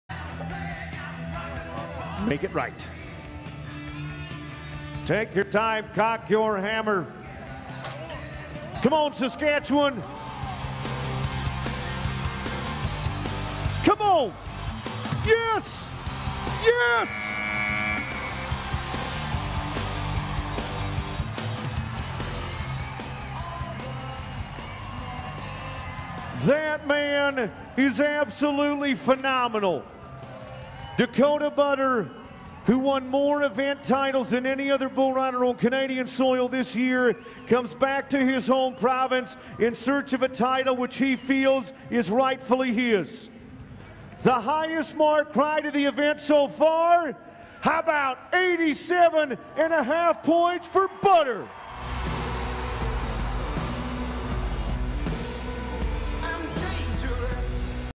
Dakota Buttar rides Finning Young Trent for 87.5 points (PBR)
at the 14th PBR Monster Energy Canadian Finals in Saskatoon, Saskatchewan at the Sasktel Centre on November 22-23, 2019.